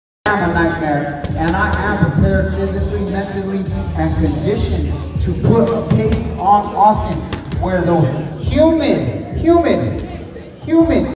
John Dodson Open Workout & Diego Sanchez Interview
Bareknuckle Fighting Championship Knucklemania Mania 3

media meet up

February 15th In Albuquerque, NM